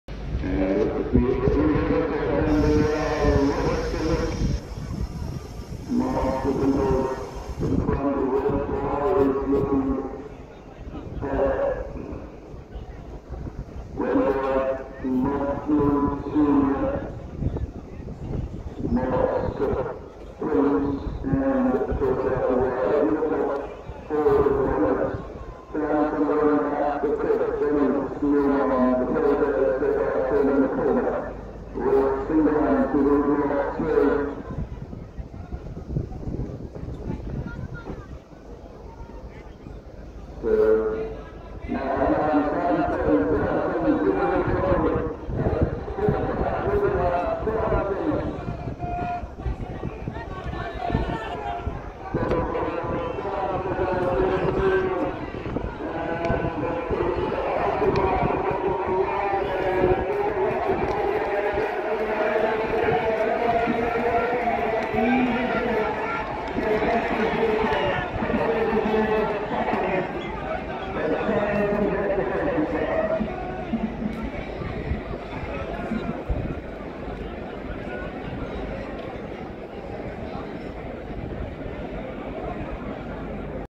1st Race 1000 Meter Winner Samson | Ridden By Jockey Bilal Ahmad Trained By Sohail Aziz
Lahore Race Club Sunday 11 December 2022